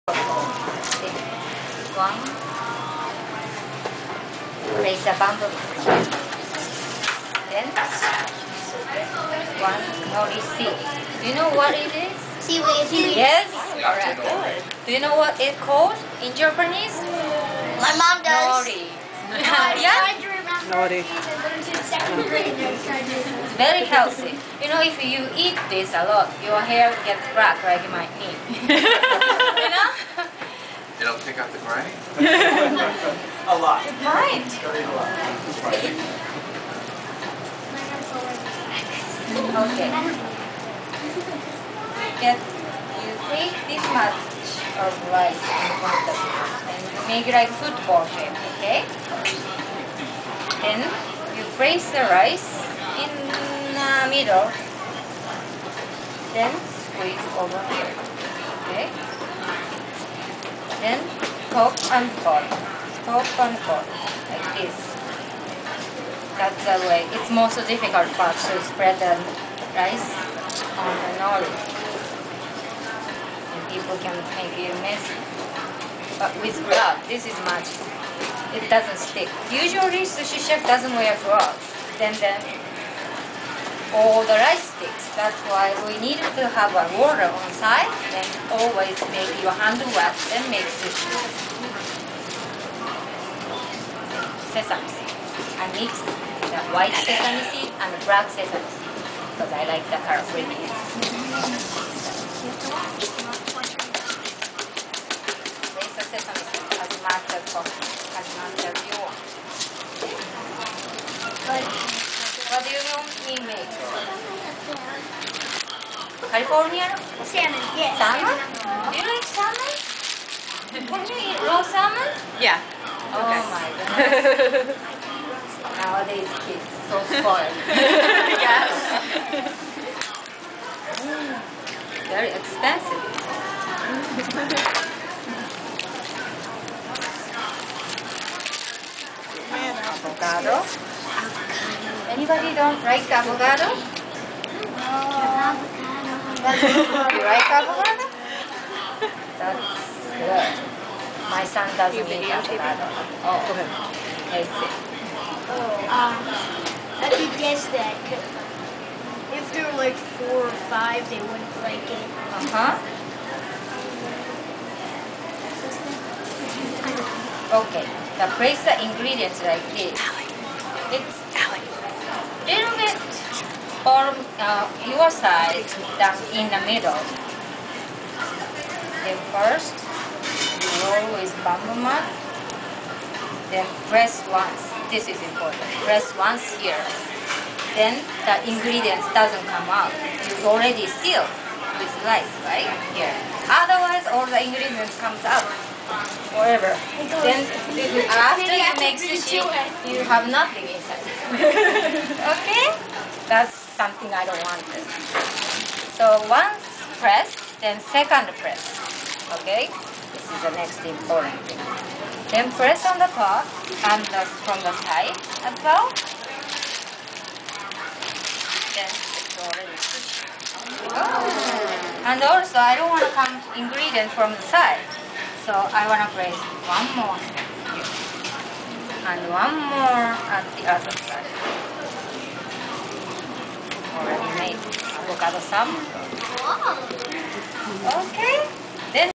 How to Make In and & Out Sushi Rolls with Executive Chef Miki Willis of Mai Cuisine: NYC Single Mom
My daughter and I went to a mommy and child sushi making class hosted by Mai Cuisine near Grand Central Station and taught by Executive Chef Miki Willis. We learned to make in and out rolls and inari